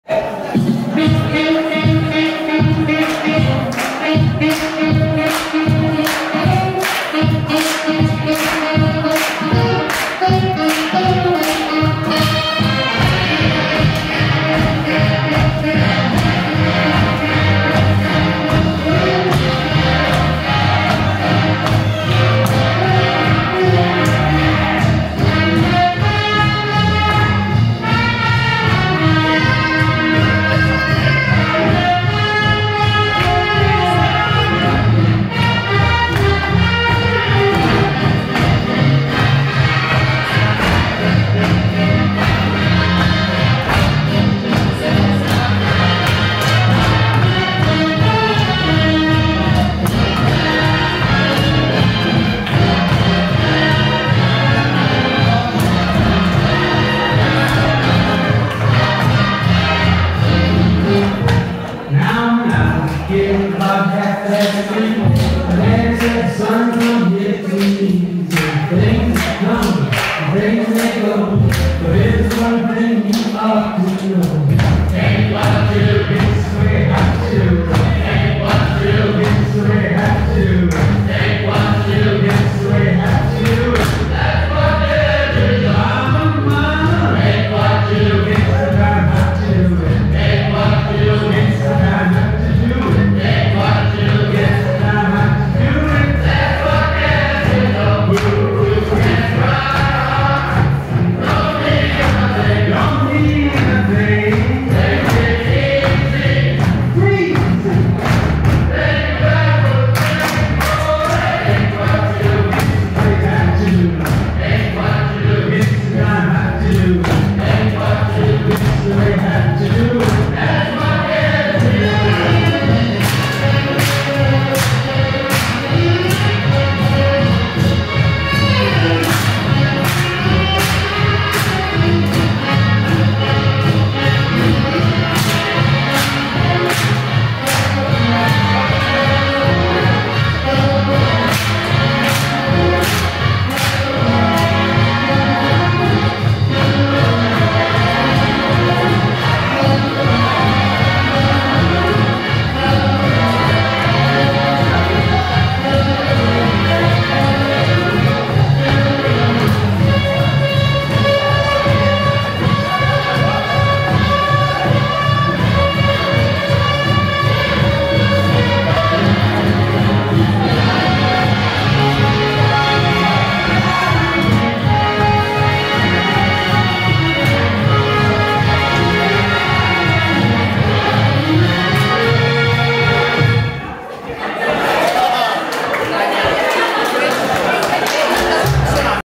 2023-08-18 The Shim Sham w/ Corner Pocket Swing @ The Pocket Social, Brisbane
An ad hoc performance by Corner Pocket Swing and guests (including members of the Brisbane Swing Dance Hub), at The Pocket Social event, Brisbane.